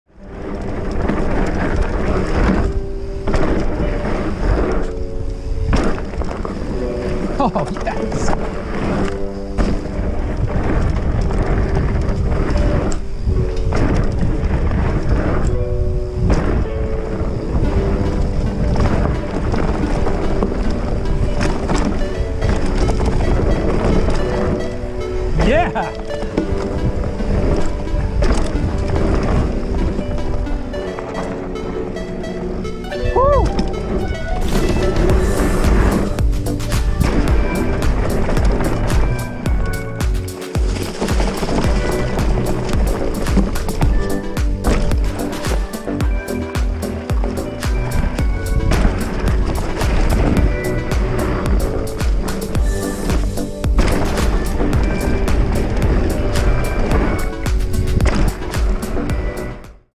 Allaire State Park ~ Jump lines 10/15/2019
A big thanks to the guys that spent time fixing up the jumps over at Allaire.  The lips are just right!